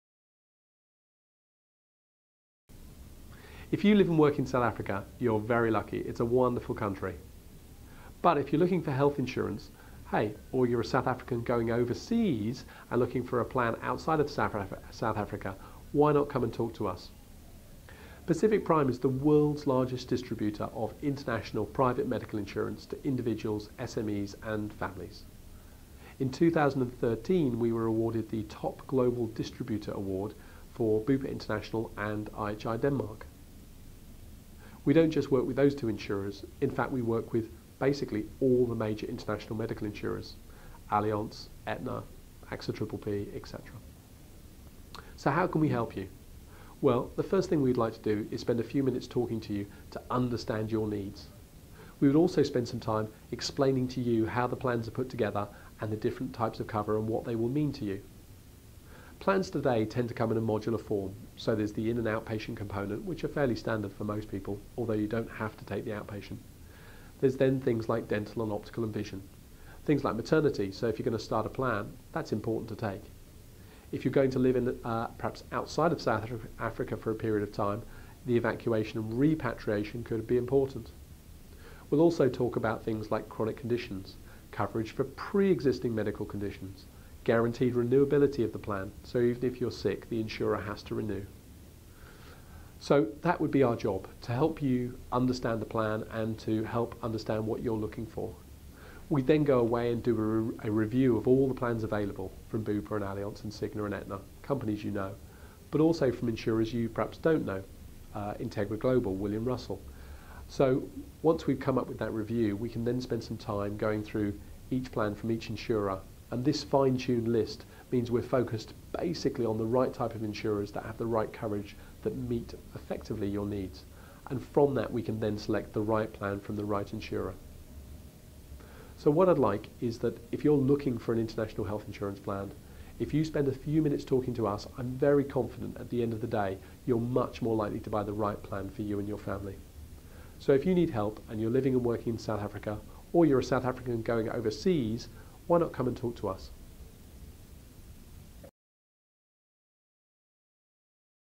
South African Health Insurance Companies
If you live or work is South Africa, you are very lucky, it is a wonderful country. But if you are looking for health insurance, or if you are a South African going overseas and looking for a plan outside South Africa, why not come and talk to us ? Pacific Prime is the world largest distributor of international private medical insurance to individuals SMEs and families. In 2013 we were rewarded the top global distributor award for Bupa international and IHI Denmark. We don't just work with these 2 insurers. In fact we work with basically all the major international medical insurers. Allianz, Aetna, Axa PPP etc. So how can we help you ? Well the first things we would like to do is to spend few minutes talking to you, to understand your needs. We will also spend some time explaining to you how the plans are good together, and the different type of cover and what they would mean to you. Plans today tend to come in a modular form, so there is an impatient component, which is fairly standard for most people, although you don't have to take the outpatient. There is then thing like dental optical or outpatient. Things like maternity so you think to start a family, that is important to take. If you are going to live perhaps outside South Africa for a period of time the evacuation and repatriation are gone to be very important. We will also talk about things like chronic conditions, coverage for pre existing medical conditions, guarantee  renewability of the plan, so if you are sick, the insurer has to renew, So that would be our job to help you understand your plan and to help understand what you are looking for. We then go away and do a review of all the plan available from Bupa and Allianz and Cigna and Aetna. Companies that you know, but also from insurance that you perhaps don't know, IntegraGlobal, William Russell, so when we come up with that review we can then spend time by going from each plan from each insurer and create you a list on which we will be focus basically on the right type of insurers with the right coverage, that mean that fit exactly to your needs, and from that we can then select the right plan from the right insurer. So what I would like is if you are looking for an international health insurance plan, if you spend a few minutes talking to us, I am very confident that at the end of the day, you are much more likely to found the right plan for you and your family. So if you need help and you are living and working in South Africa or you are a South African going overseas why not coming and talk to us ?